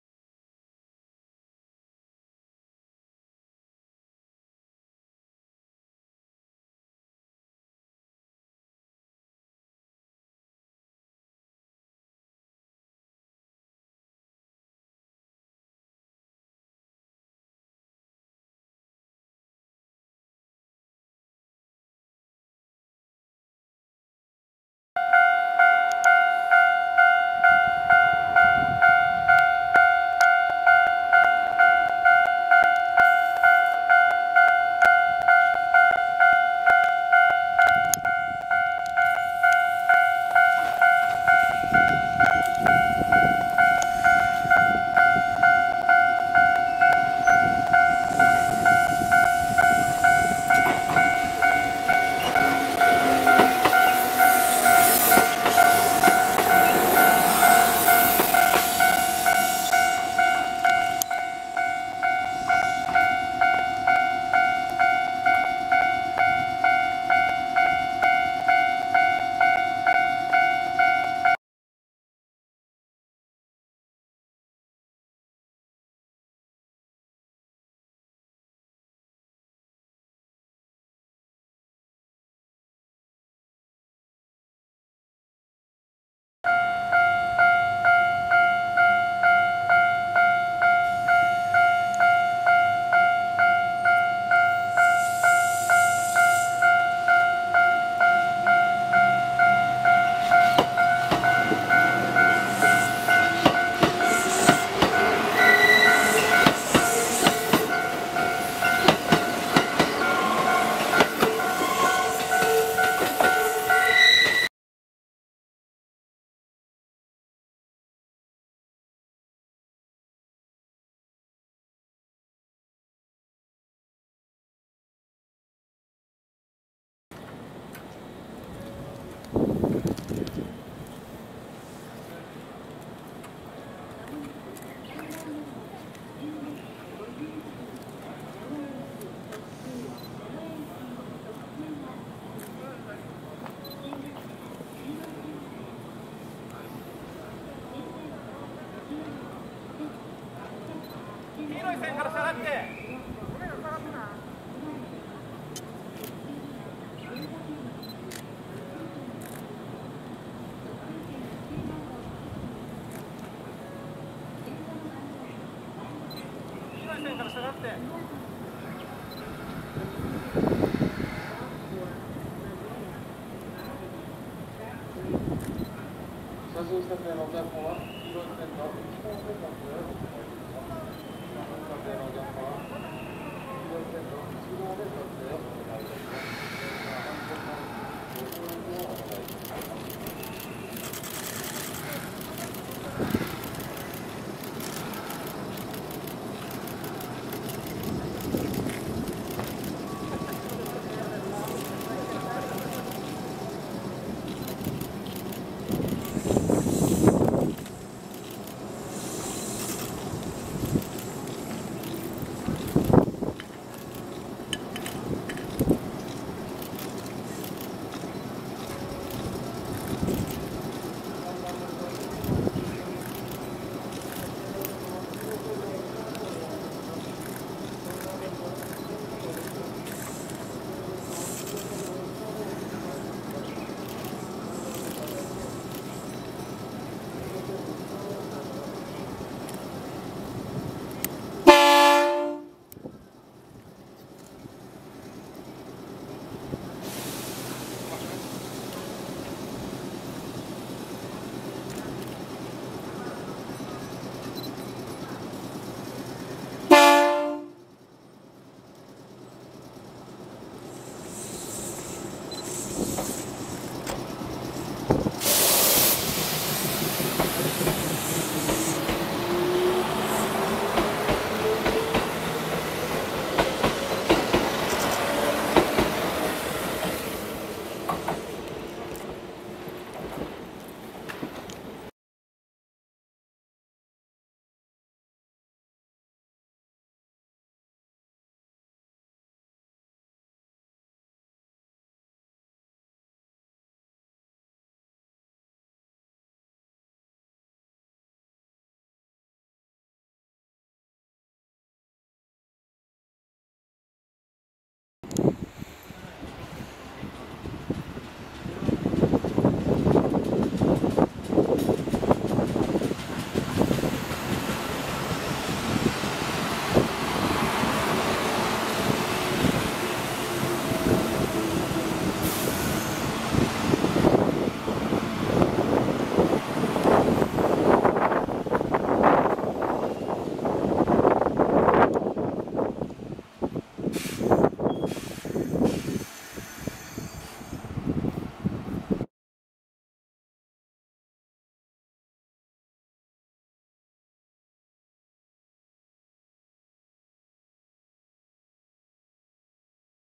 【近鉄特急】ありがとう12200系「スナックカー」@伊勢志摩
4/25 お別れ運転を行っている12200系「スナックカー」を記録しました。
0:00 山田線・斎宮駅
0:20 志摩線・志摩赤崎駅
2:00 鳥羽線・五十鈴川駅
4:55 志摩線・志摩磯部&大阪線・高安駅